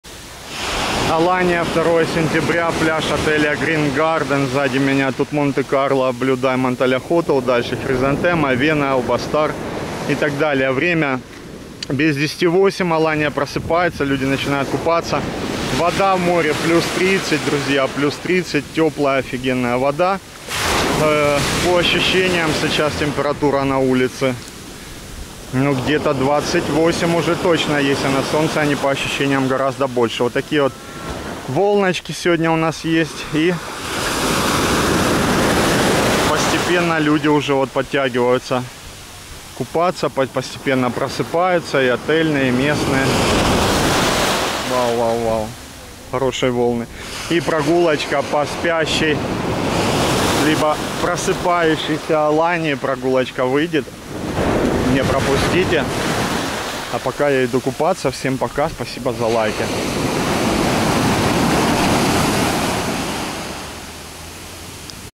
ALANYA 2 СЕНТЯБРЯ ПОГОДА И МОРЕ GREEN GARDEN ПЛЯЖ
🇹🇷 Алания утро 2 сентября море погода пляж.
Сервис этот принимает рубли, гривны, евро, доллары.
Буду премного благодарен каждому неравнодушному зрителю.